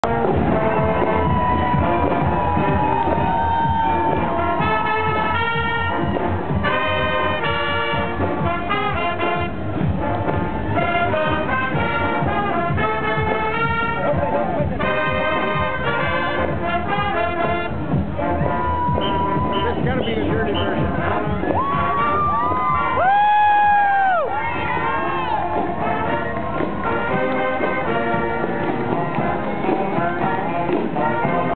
San Francisco Gay Freedom Marching Band
San Francisco Gay Freedom Marching Band performing at SF Pride Parade 2008